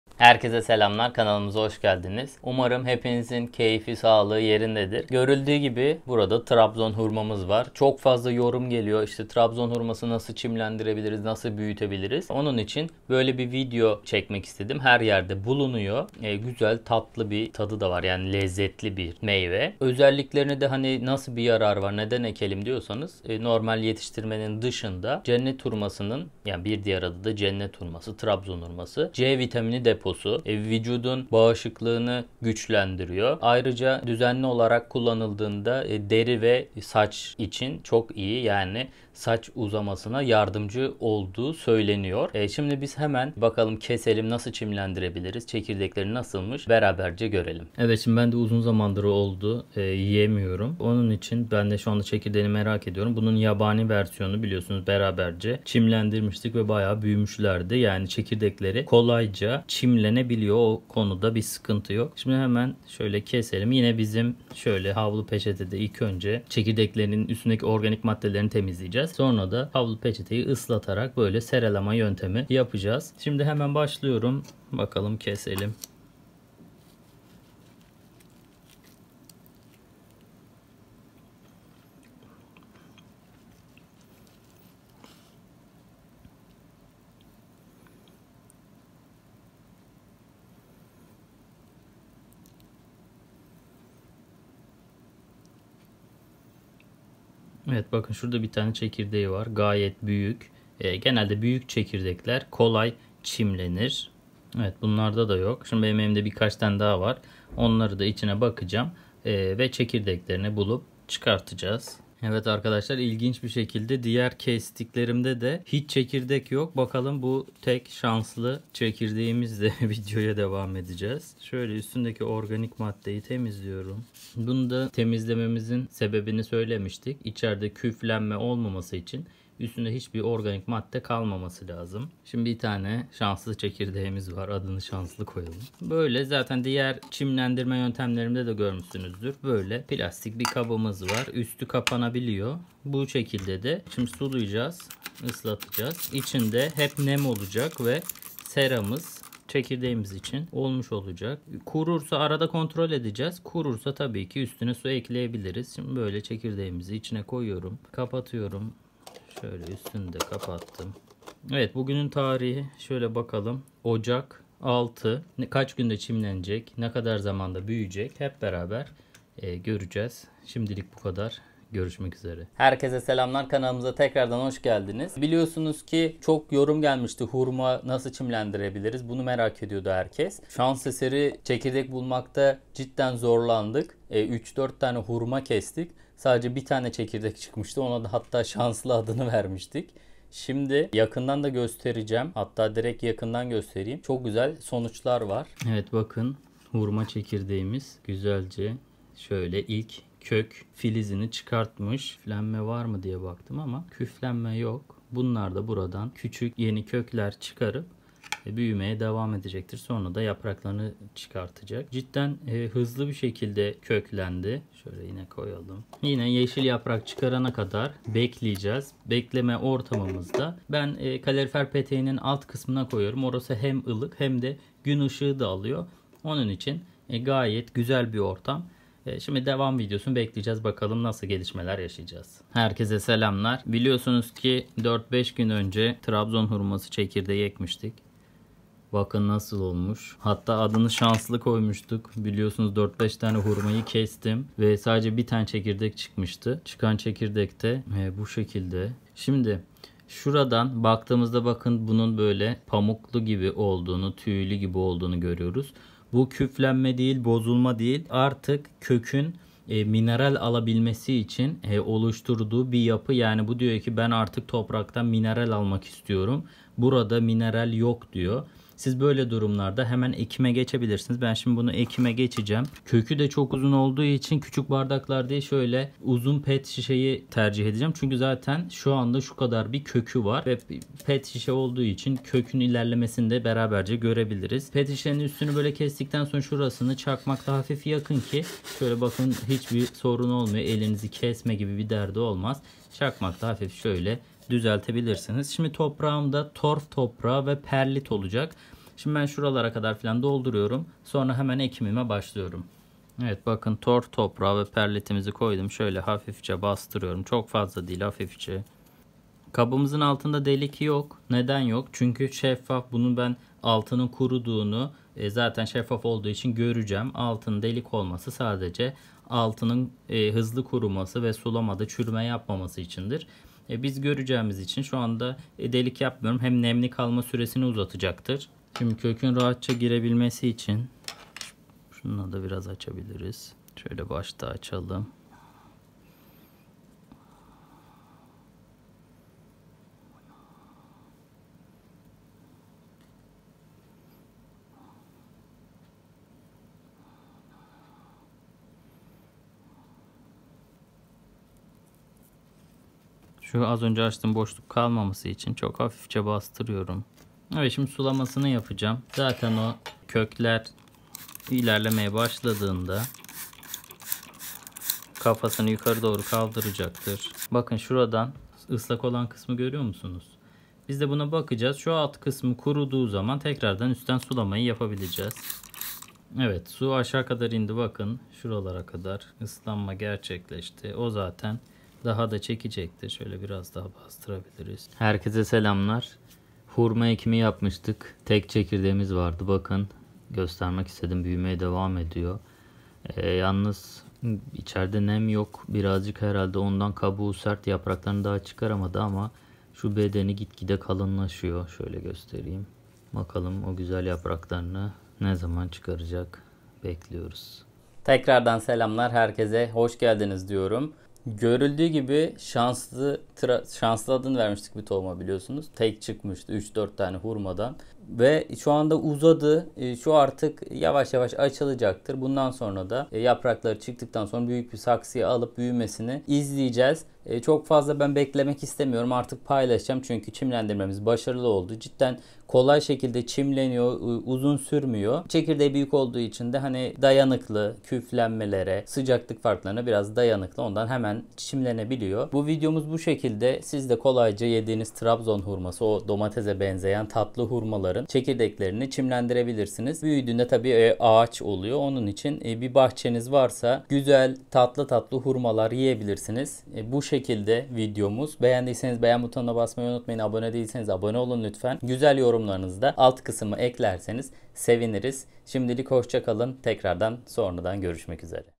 Trabzon Hurması Çimlendirme
Herkese selamlar, bu videoda domatese benzeyen tatlı mı tatlı bir meyve ekeceğiz tahmin ettiniz evet. Cennet hurması bir diğer adıyla trabzon hurması. Trabzon hurmasının faydaları saymakla bitmez harika bir tadıda var ama bizi ilgilendiren kısmı şimdilik trabzon hurması çekirdekten nasıl yetiştirilir trabzon hurması ağacı nasıl yetiştirilir. Gerçekten çok hızlıca büyüyen bir bitki. Olta mühendisinden sizlere trabzon hurmasını nasıl kolay yetiştirirsiniz taktikler burada iyi seyirler...

"Bitki Mühendisi"ne destek olmak için  "beğen" ve  "yorum bırak" ilginiz için teşekkür ederiz.✨